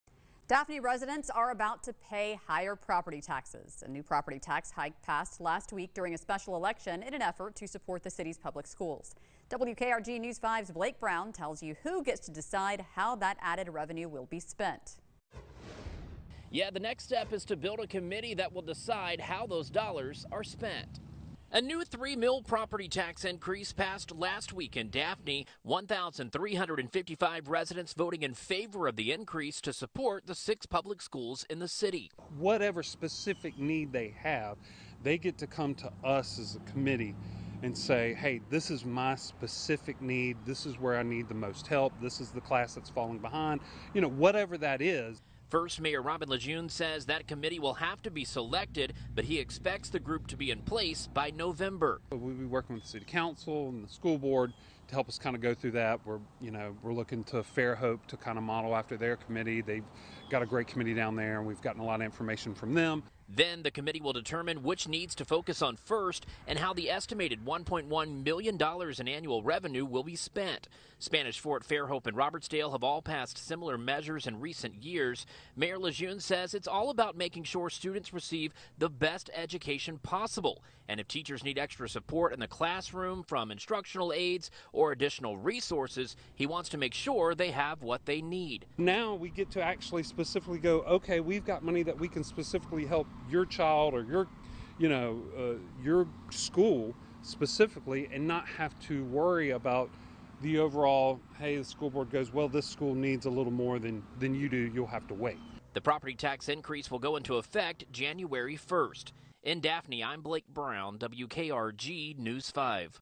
New committee will oversee property tax revenue for Daphne schools
A new 3-mill property tax increase passed last week in Daphne. 1,355 residents voted in favor of the increase to support the 6 public schools in the city.